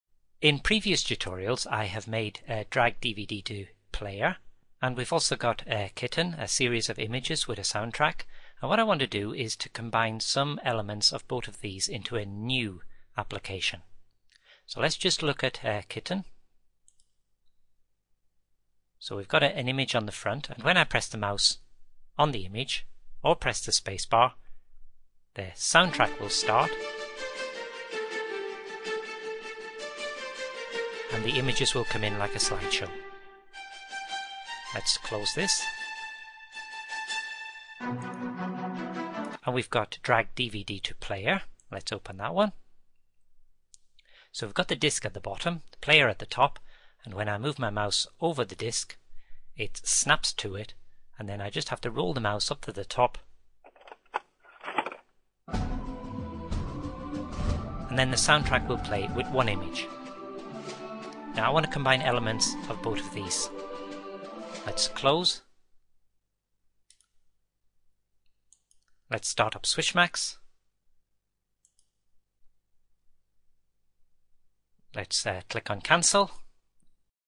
Swishmax Tutorials - making an early mouse activity - Part 1.
Making an early mouse activity which can be used by tots, preschool kids, early years and special needs education. The activity/game will help those using the mouse for the first time. It will require no clicking or dragging just moving the mouse for a fun reward